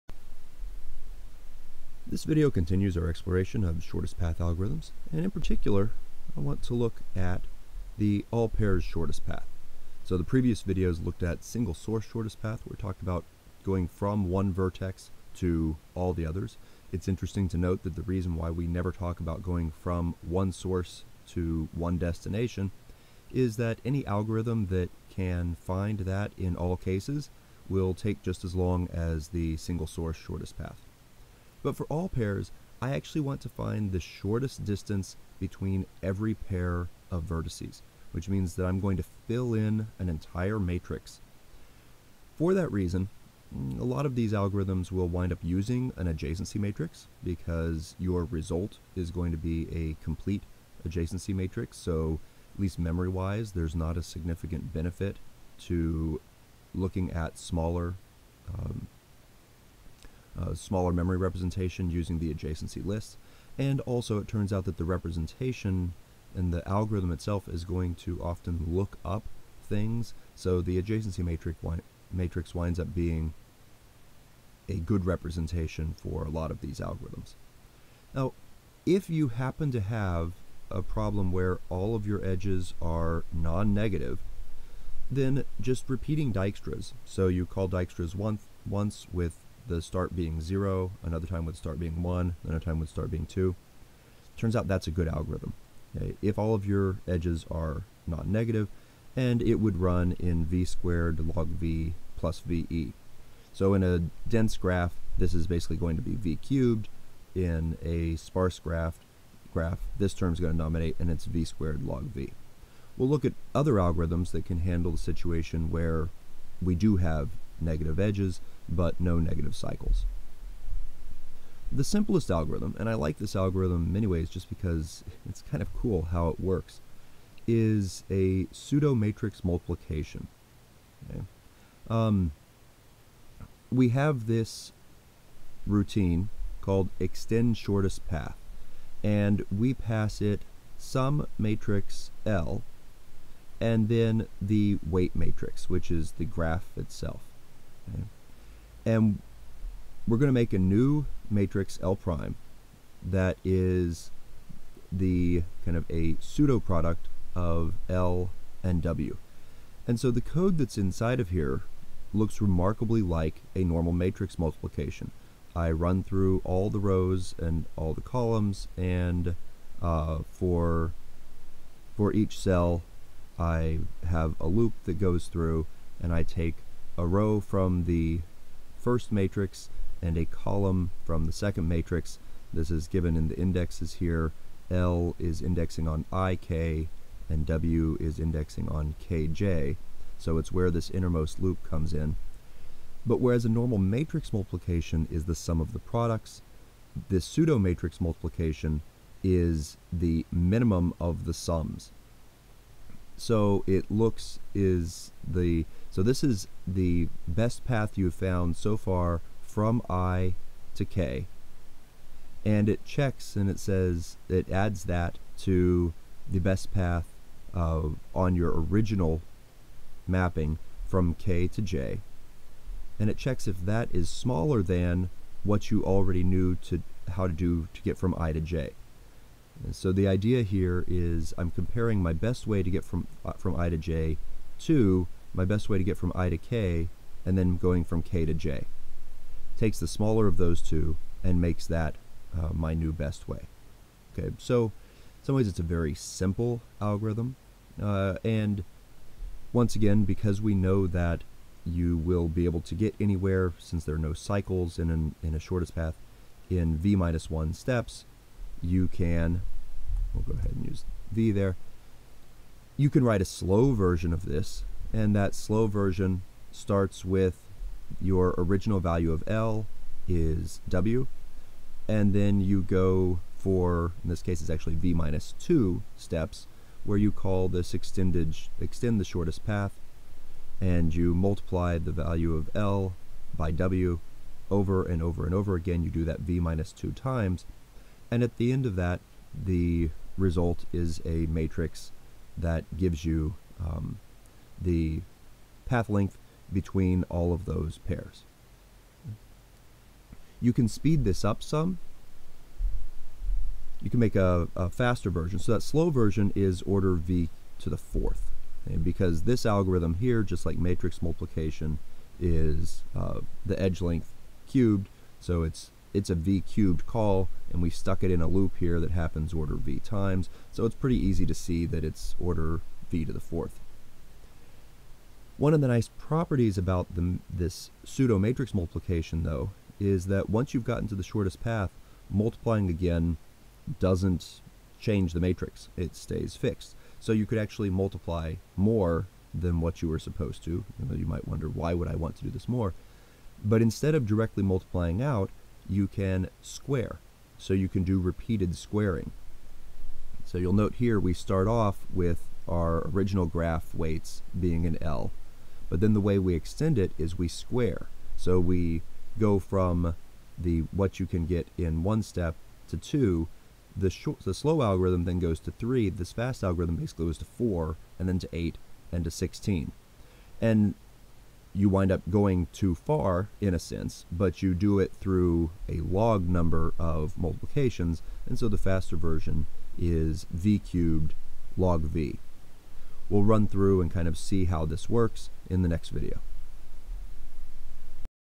All-Pairs, Shortest Path - Pseudo Multiply
This video describes the idea of all-pairs, shortest path algorithms, and looks at a simple solution that uses an algorithm that resembles matrix multiplication.